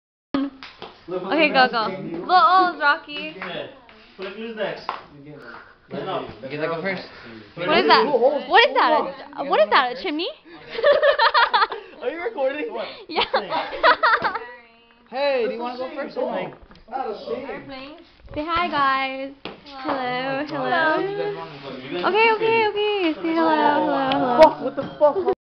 Chimney O's
Rocky smoking hookah hahaha and everyone else